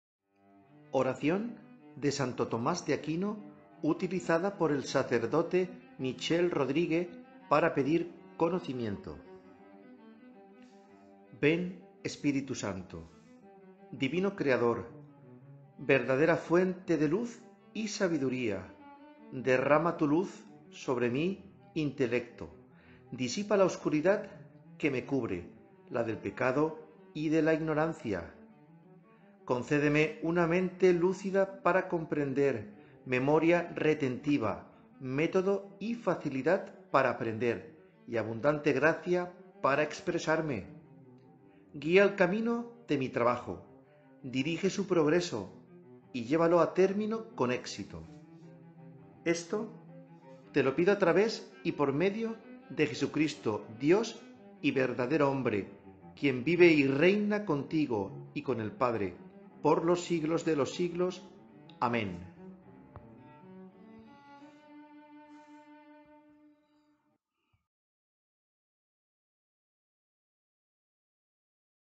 Oración de Santo Tomás de Aquino utilizada por el sacerdote Michel para adquirir conocimiento
Oración al Espíritu Santo de  Santo Tomás (para realizar antes de dar inicio al estudio).
“Ven Espíritu Santo, Divino Creador, verdadera fuente de luz y sabiduría, derrama tu luz sobre mí intelecto, disipa la oscuridad que me cubre del pecado y de la ignorancia. Concédeme una mente lúcida para comprender, memoria retentiva, método y facilidad para aprender y abundante Gracia para expresarme, guía el camino de mi trabajo, dirige su progreso y llévalo a término con éxito. Esto te lo pido a través y por medio de Jesucristo, Dios y verdadero hombre, quien vive y reina contigo y con el padre, por los siglos de los siglos.  Amén.”

@¿QUIÉN ES EL ESPÍRITU SANTO DE BIEN Y VERDAD?